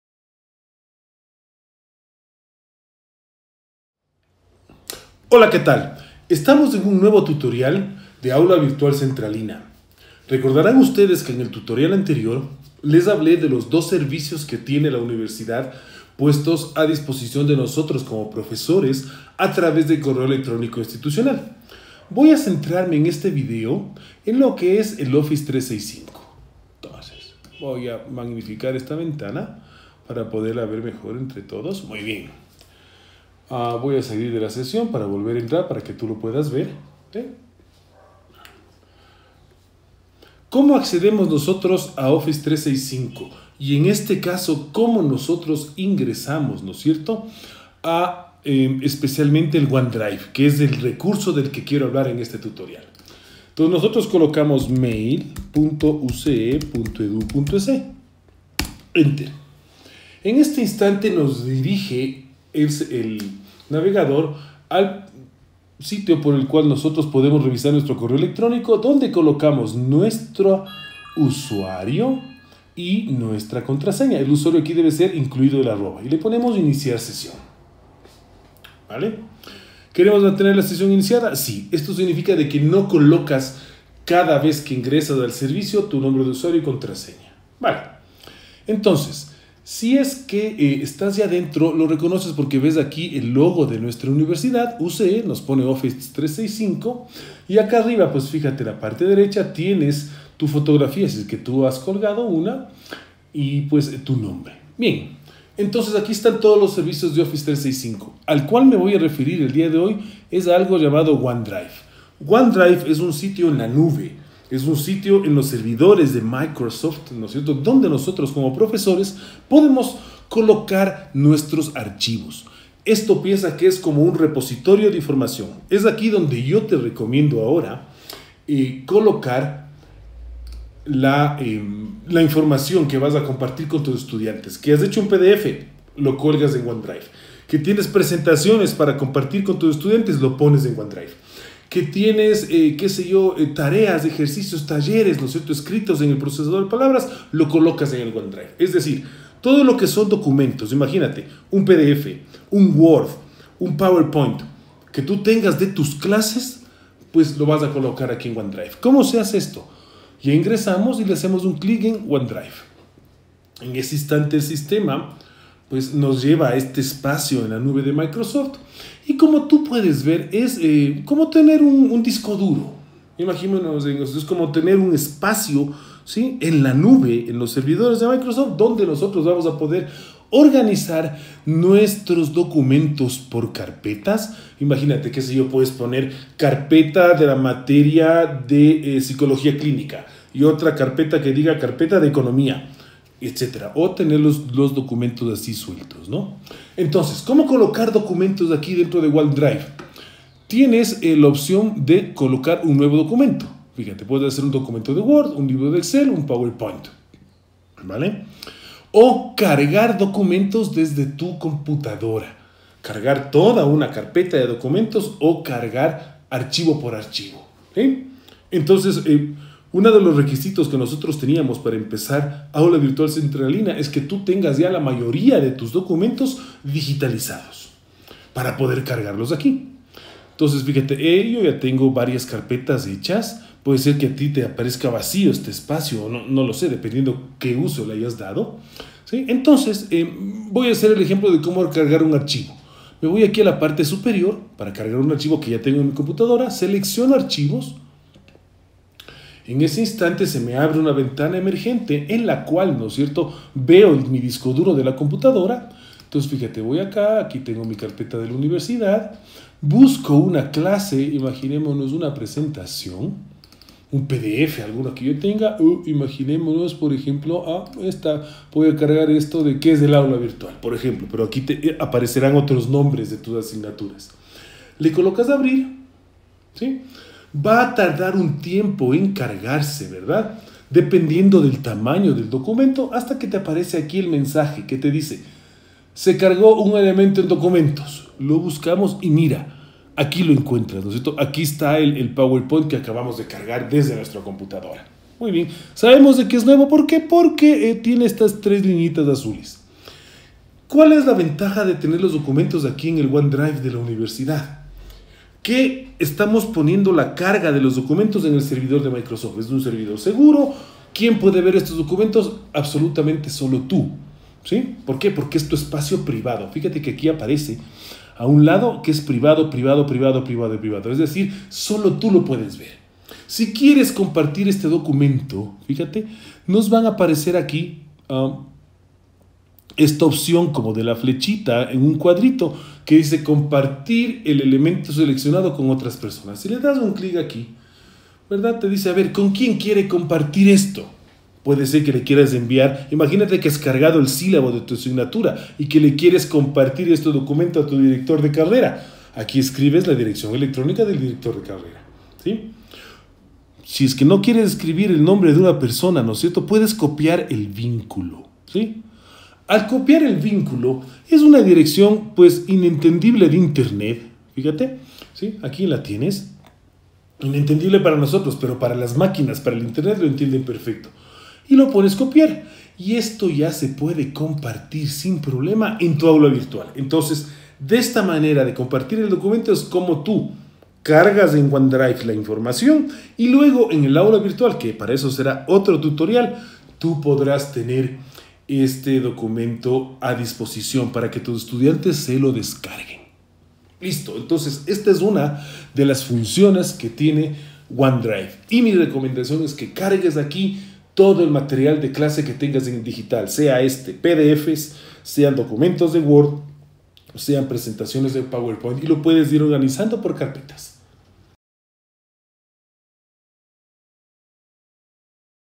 Video2. ¿cómo cargar archivos a OneDrive?
Con este recurso muestro los pasos a seguir para cargar archivos (Documentos de texto, presentaciones, PDFs) al espacio de OneDrive que la UCE pone a disposición de los profesores de la UCE para enfrentar la crisis sanitaria que vivimos en Ecuador